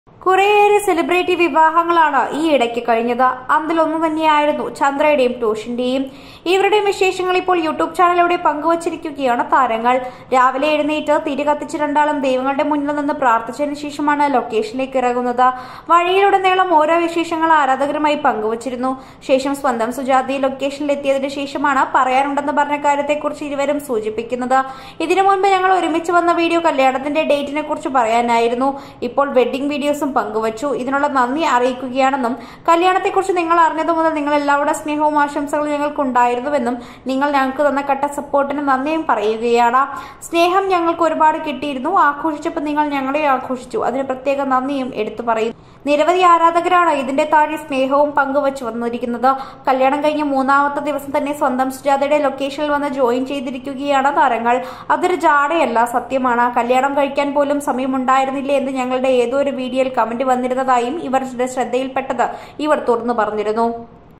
ചന്ദ്ര ലക്ഷ്മണും ടോഷ് ക്രിസ്റ്റിയും മൂന്നാമത്തെ ദിവസം തന്നെ വീടുവിട്ടിറങ്ങി... l Chandra Lakshman
കല്യാണം കഴിഞ്ഞ് മൂന്നാമത്തെ ദിവസം തന്നെ ചന്ദ്ര പോയി..